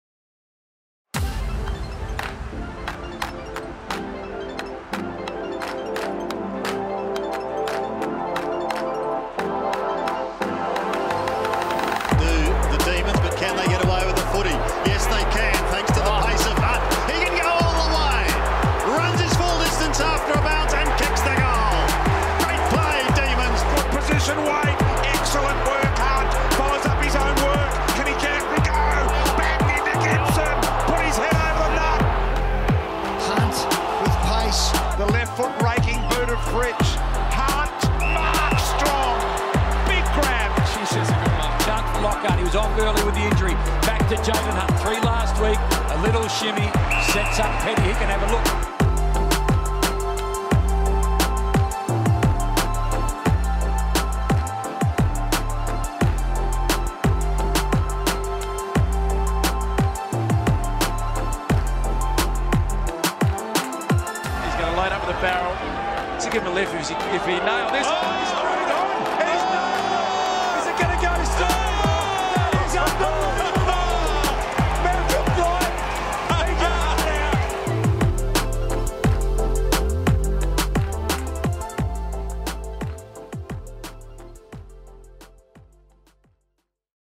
Speedster Hunt flies West
Unrestricted free agent and former Melbourne Demon Jayden Hunt signs with the Eagles ahead of the 2023 season